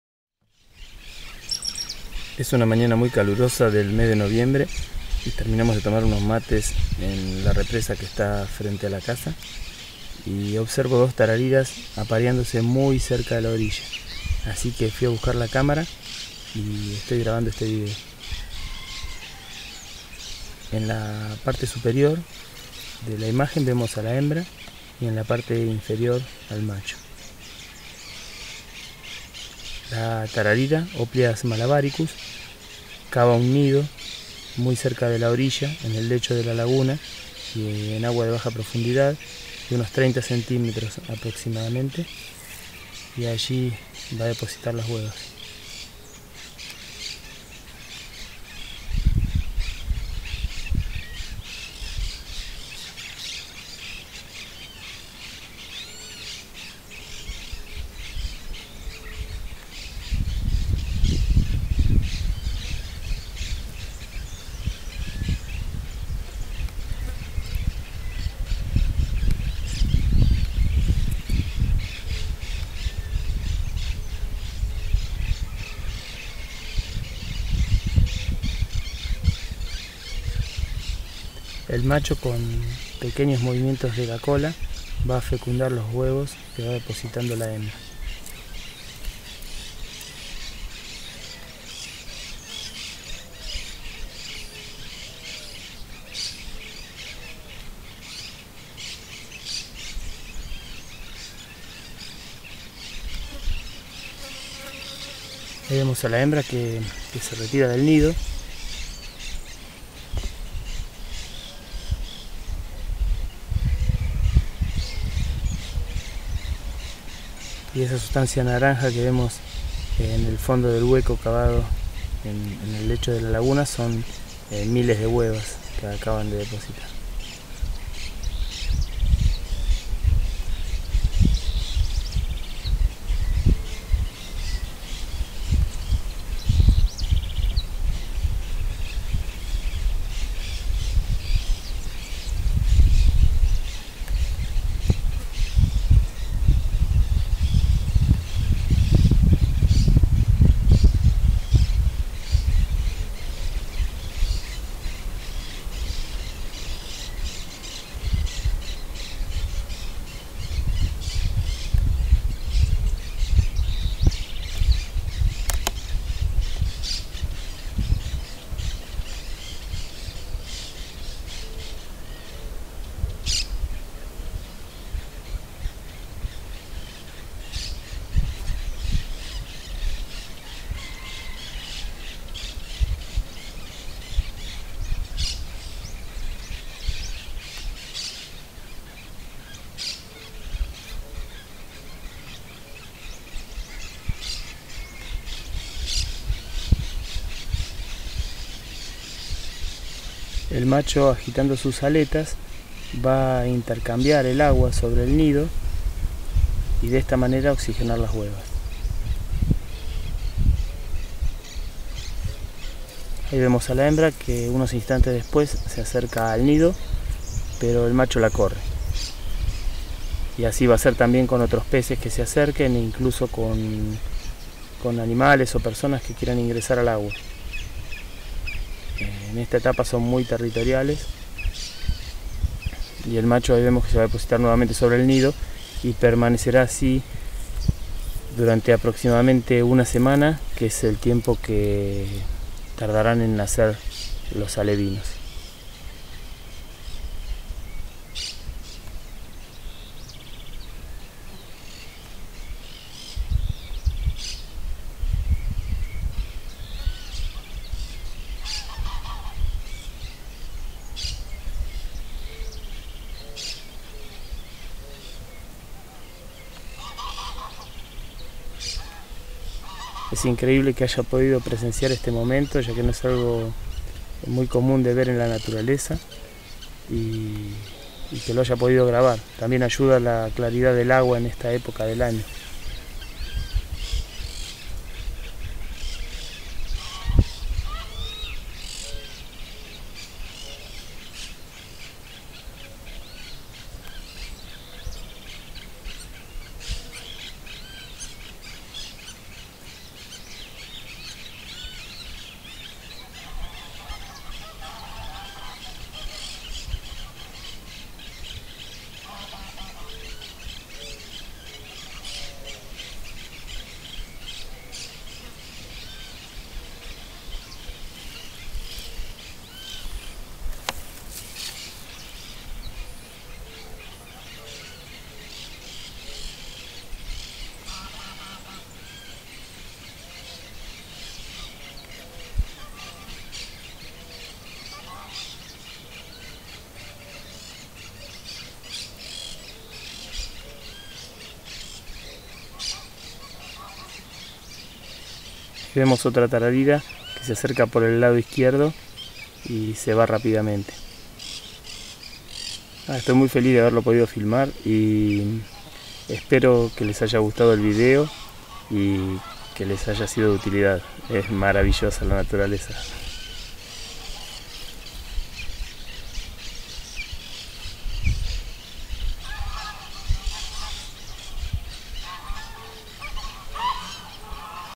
Tararira reproduccion, apareamiento y desove.
Tarariras apareandose y desovando en su nido en mi represa.